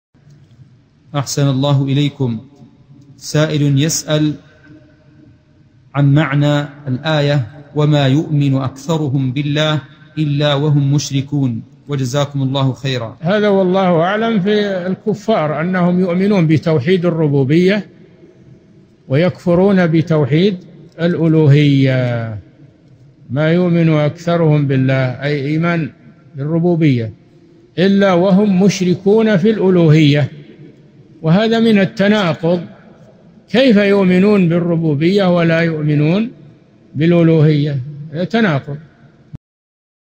[50 /84] سائل يسأل عن معنى الآية: { وما يؤمن أكثرهم بالله إلا وهم مشركون } ؟| الشيخ صالح الفوزان
[50 /84] السؤال: سائلٌ يسأل عن معنى الآية: { وما يؤمن أكثرهم بالله إلا وهم مشركون } ؟
 الجواب: هذا والله أعلم في الكفار أنهم يؤمنون بتوحيد الربوبية ويكفرون بتوحيد الألوهية {وما يؤمن أكثرهم بالله} أي إيمان بالربوبية {إلا وهم مشركون} في الألوهية، وهذا من التناقض كيف يؤمنون بالربوبية ولا يؤمنون بالألوهية؟ هذا تناقض."
السيرة الذاتية :
للاشتراك في قناة الشيخ صالح الفوزان
أسئلة وأجوبة من شرح كتاب السنة من سنن أبي داود (84) مقطع

للاستفسار والإقتراحات والدعم
رئيس الفريق
لمشروع كبار العلماء